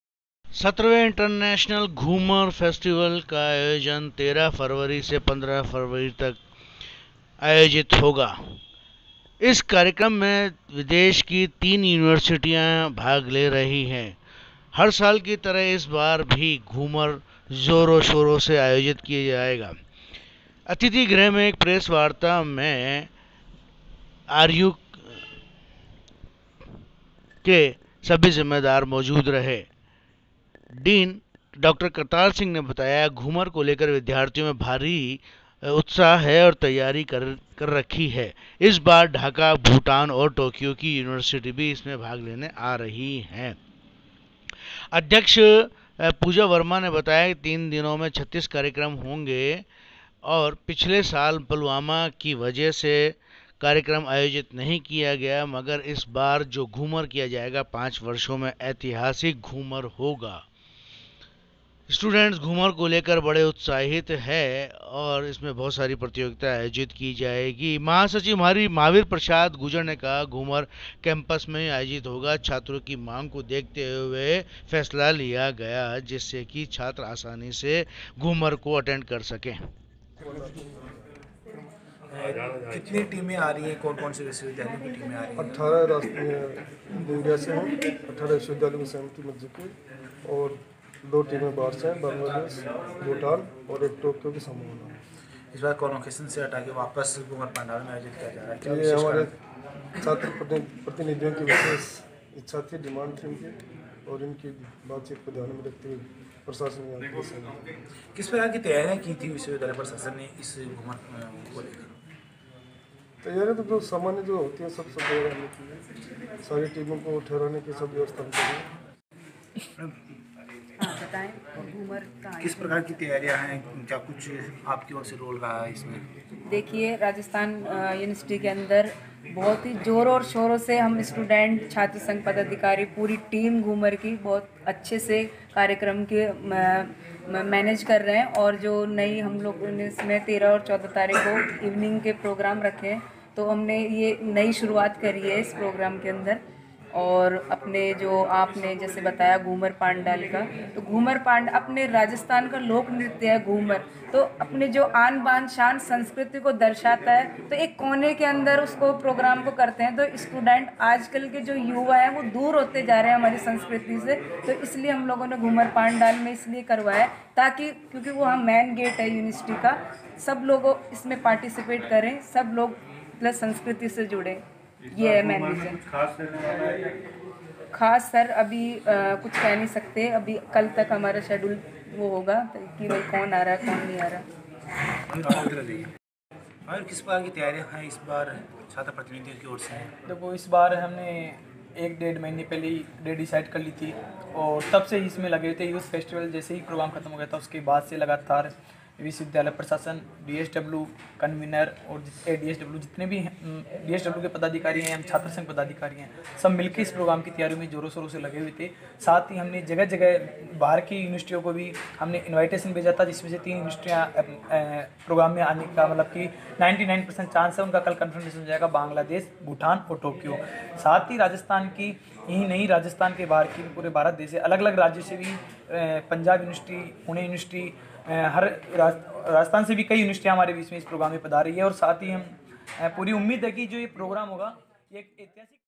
3 दिन के घूमर सस्ती बल में पहुंचेंगे देश-विदेश की यूनिवर्सिटी
जयपुर
 13 फरवरी से 15 फरवरी तक राजस्थान यूनिवर्सिटी की ओर से17वा इंटरनेशनल यूथ फेस्टिवल घूमर का आयोजन किया जा रहा है घूमर को लेकर छात्र-छात्राओं में काफी उत्साह है
प्रेस वार्ता में डॉक्टर करतार सिंह ने बताया विदेश विदेश से बड़ी संख्या में यूनिवर्सिटी आ रही है और छात्रों में काफी उत्साह है इसकी तैयारियां पूरी कर ली गई है 3 दिन के फेस्टिवल में कई कार्यक्रम आयोजित होंगे
राजस्थान विश्वविद्यालय छात्र संघ की अध्यक्ष  पूजा वर्मा ने बताया कि पिछले साल पुलवामा पर हमले होने की वजह से घूमर का आयोजन नहीं हो पाया था मगर इस बार जो घूमर होगा वह इंटरनेशनल लेबल का होगा और इसके अलावा 5 वर्षों में कभी ऐसा घूमर नहीं हुआ होगा
महासचिव महावीर प्रसाद गुर्जर ने कहा इस बार कैंपस में ही घूमर का आयोजन किया जाएगा जिससे छात्र छात्राएं उसका लुत्फ उठा सकें